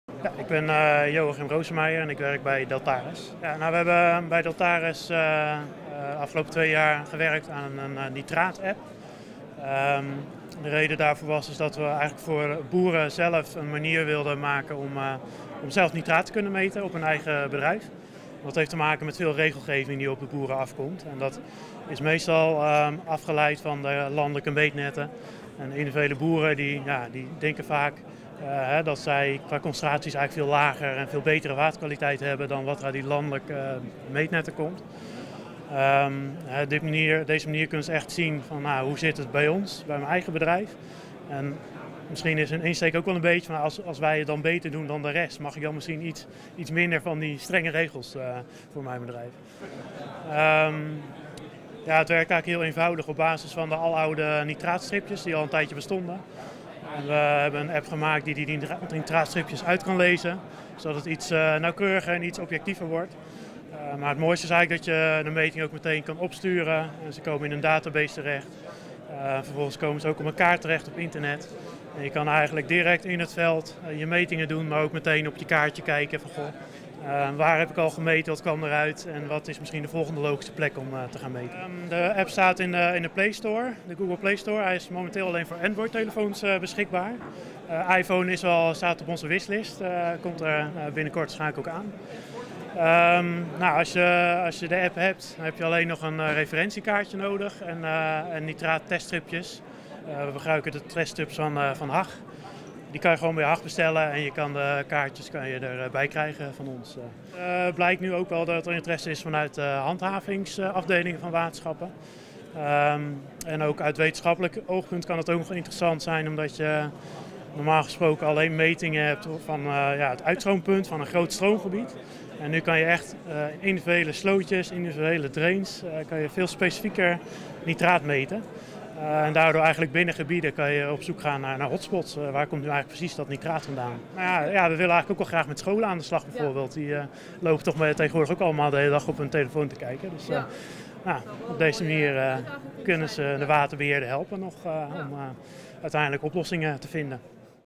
Waterinfodag 2017 | Interview 17 - Joachim Rozemeijer - Deltares
Interview opgenomen op de Waterinfodag 2017 - Jaarlijks congres over de informatievoorziening in de watersector - Congrescentrum 1931 - Den Bosch - Donderdag 30 maart 2017